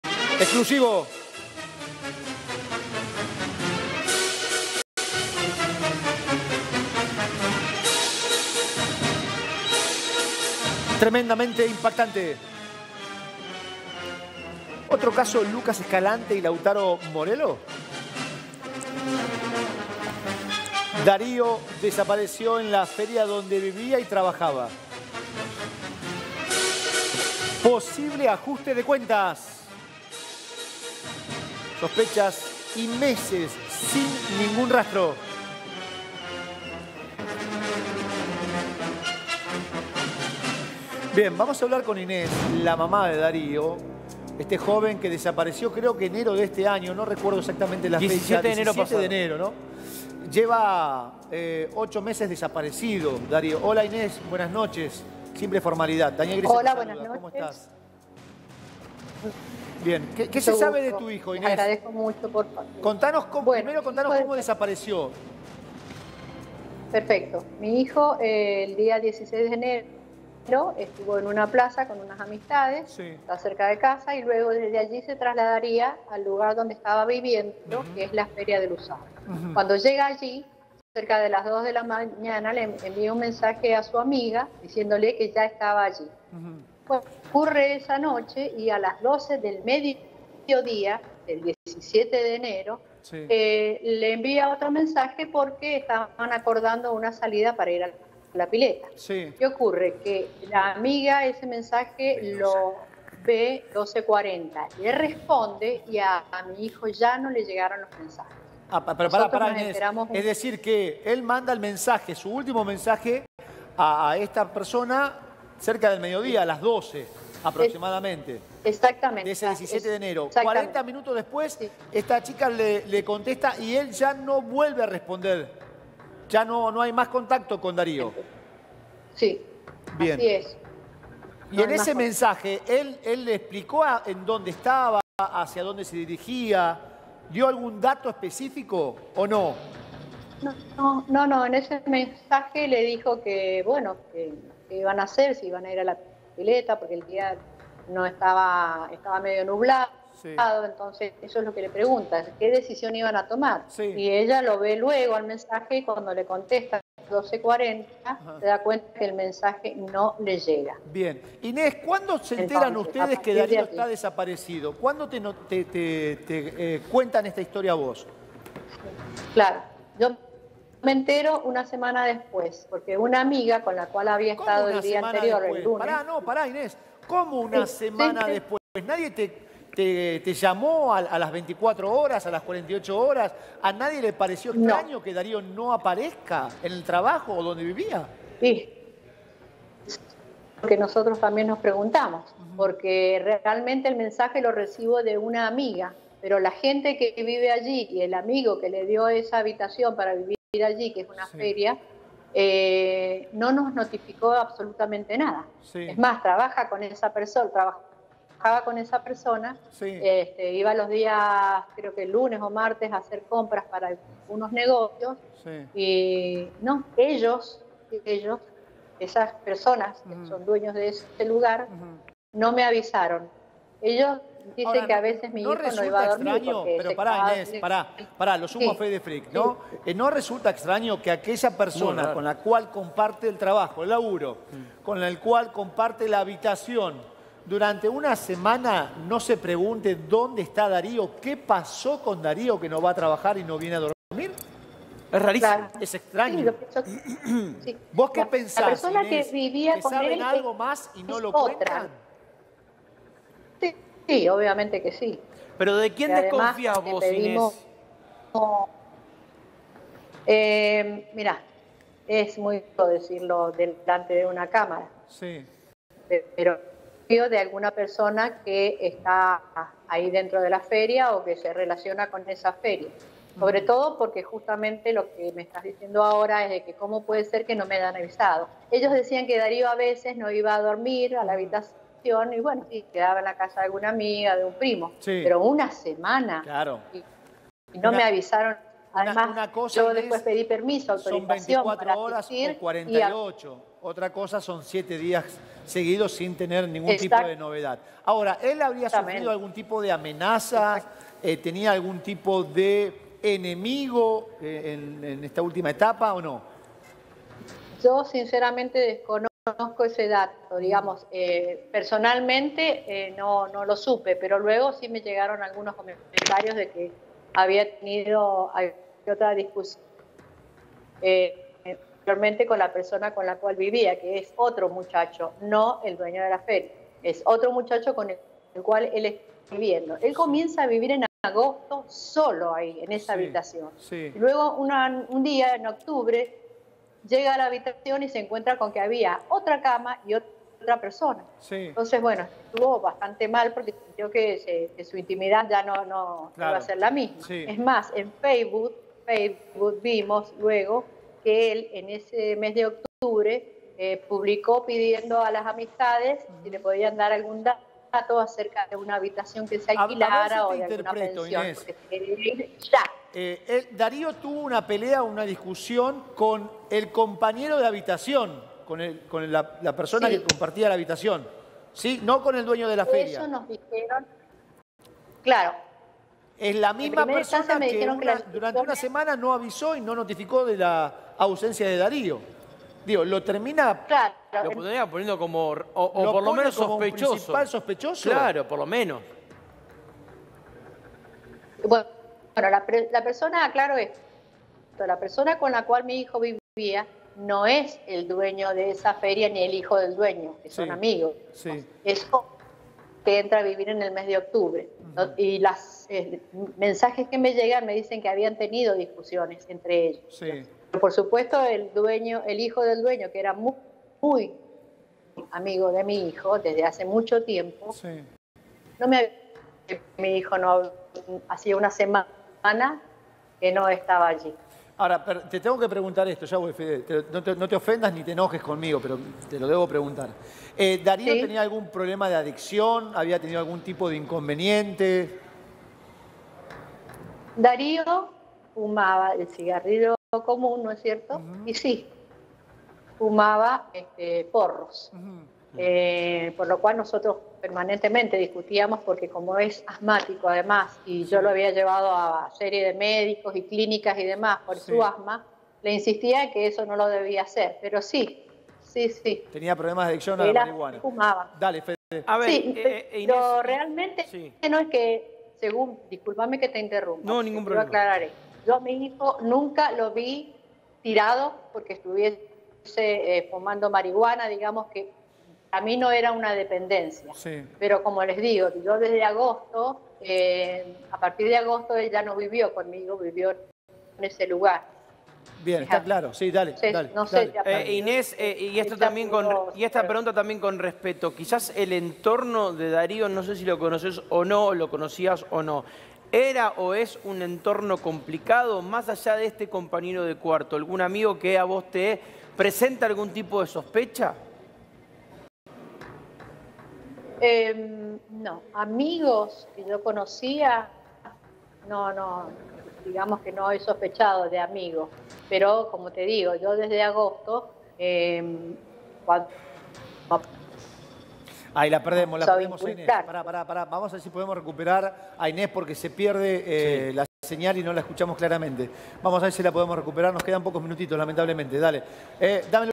¿Otro caso Lucas Escalante y Lautaro Morello? Sospechas y meses sin ningún rastro
Daniel Gretzchel habló con Inés, mamá de Dario, chico que está desaparecido desde el 17 de enero. Ese día, Dario le mandó mensajes a una amiga para ir a la pileta, pero luego de ahí no hubo más noticias de el.